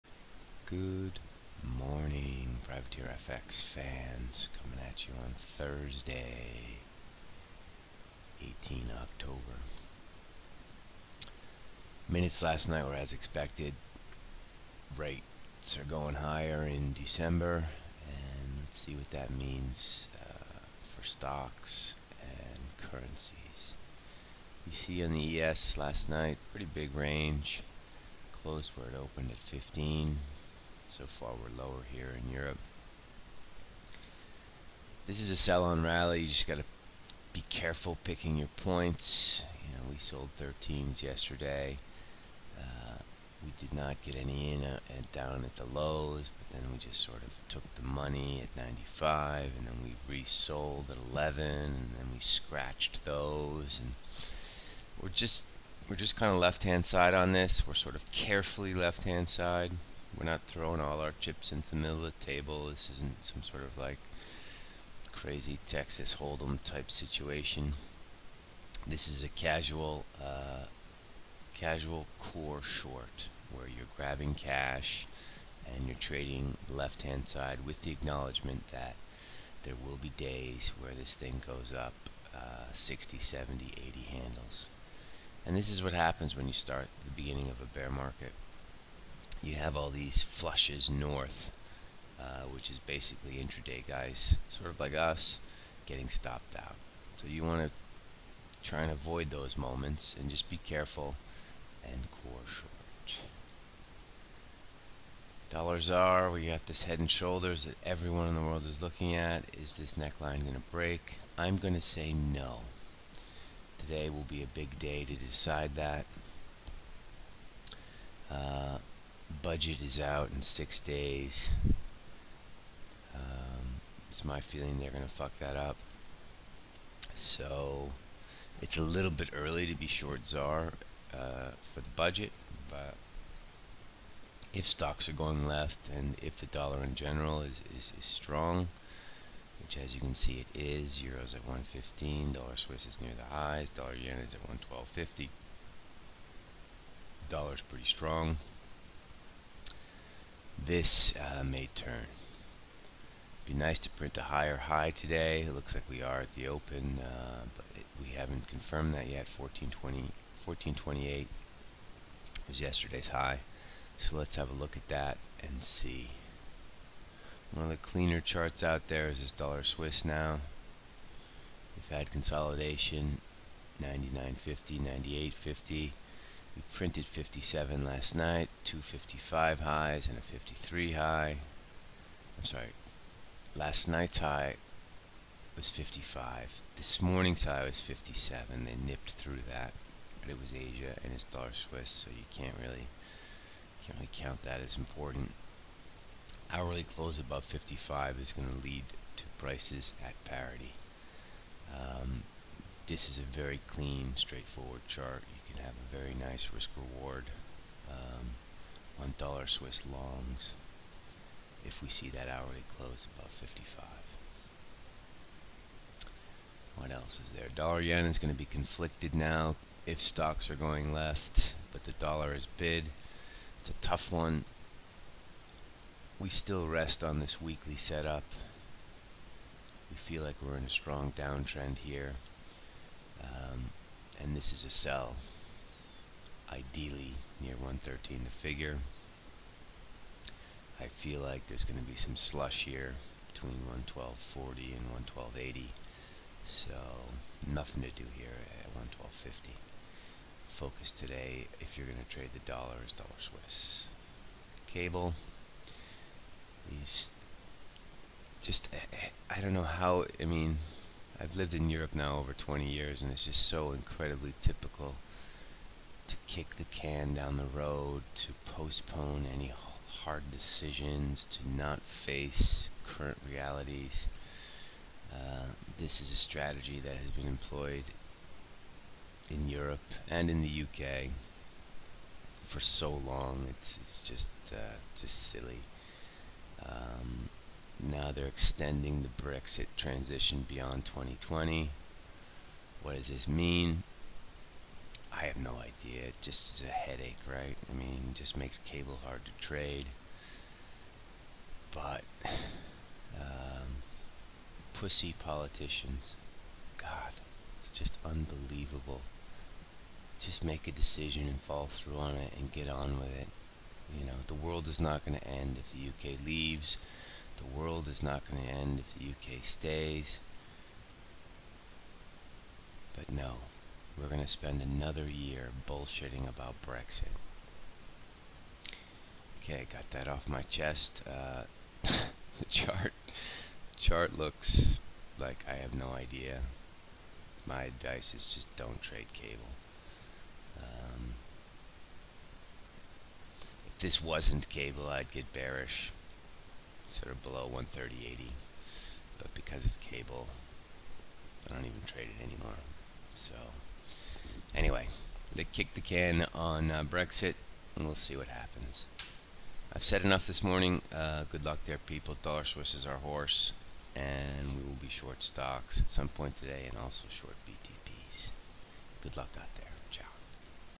18Oct18 Traders Opening Call - PrivateerFX
Our view on today's Open in the FX & Crypto markets.

Web: PrivateerFX.com

THIS WAS
18Oct18 Traders Opening Call - PrivateerFX